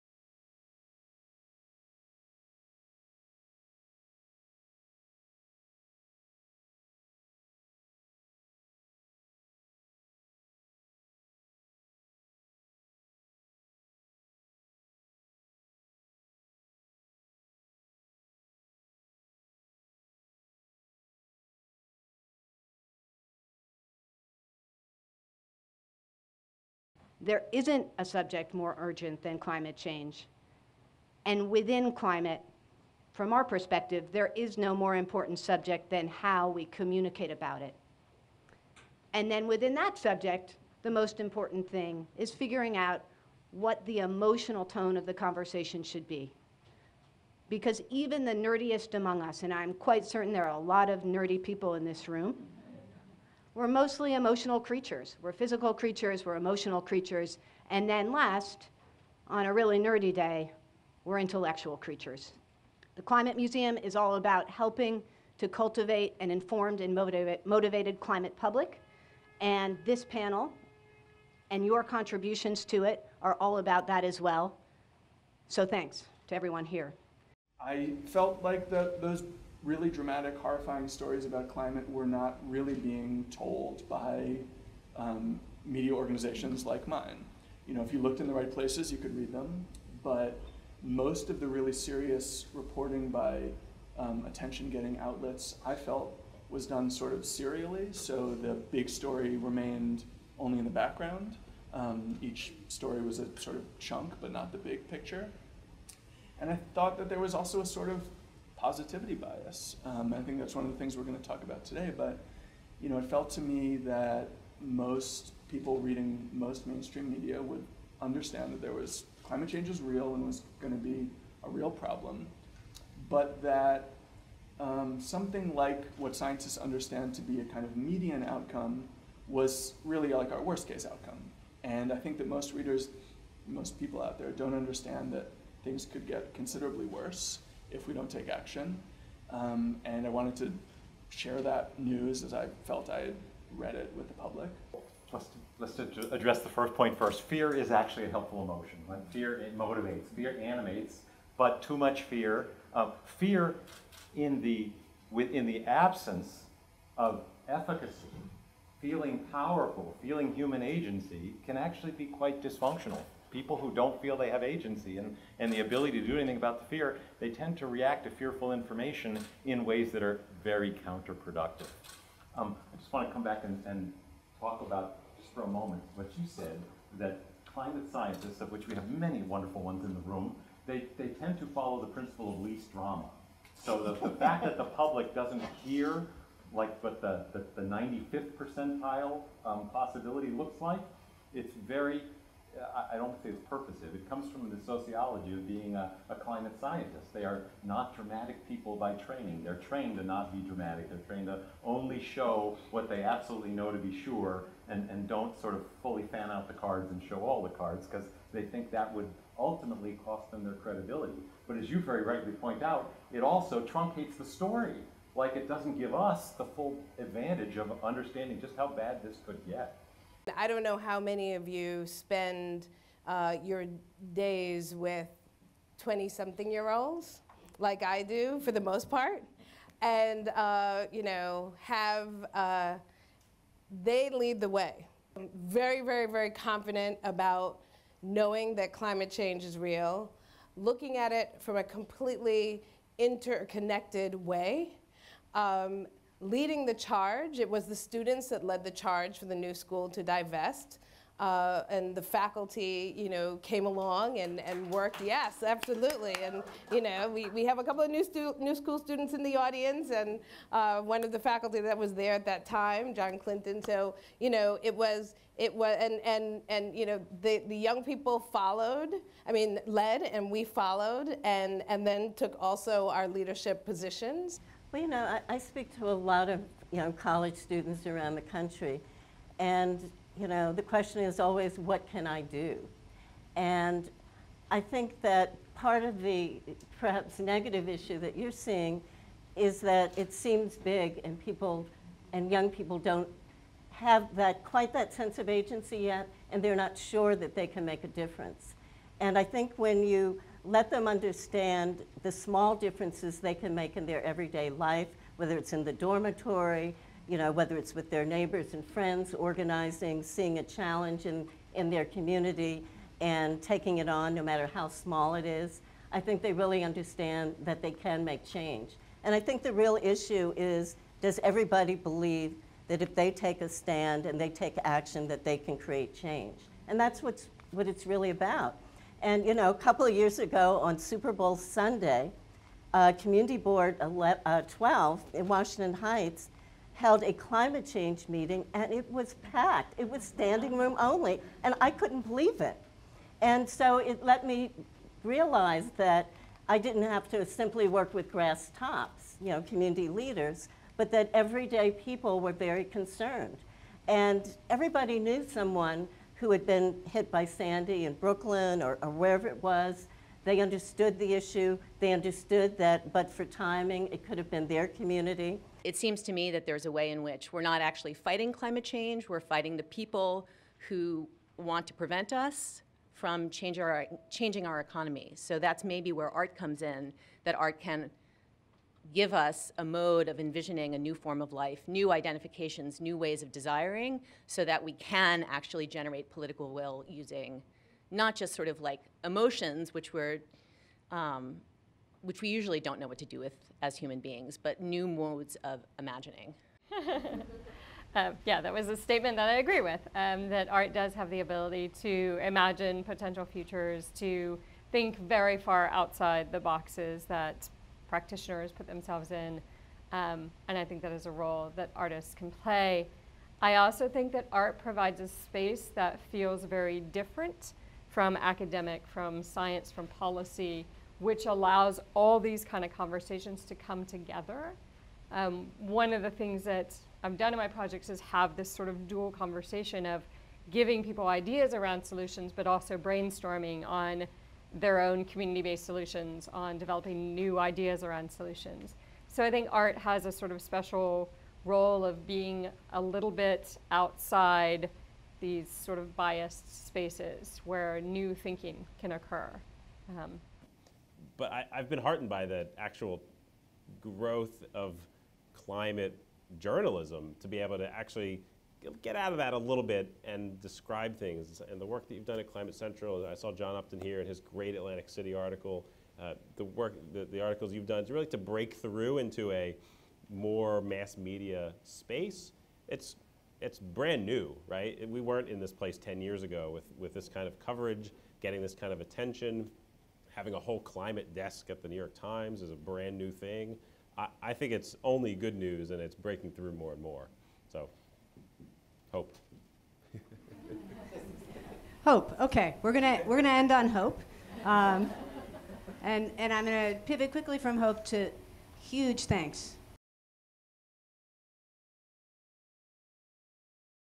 Panel: Fear and Hope in the Climate Conversation
In Fall 2017 Climate Week convened a panel of experts across the disciplines to discuss the role of emotions in building climate dialogue.

With:
Miranda Massie, Climate Museum, Host
Heidi Cullen, Climate Central, Moderator
Michelle DePass, The New School
Ed Maibach, Center for Climate Change Communication at George Mason University
Eve Mosher, Artist
Peggy Shepard, WE ACT for Environmental Justice
David Wallace-Wells, New York Magazine

Around the time of our founding, before we launched public programming, the media coverage we received included this piece in the New Yorker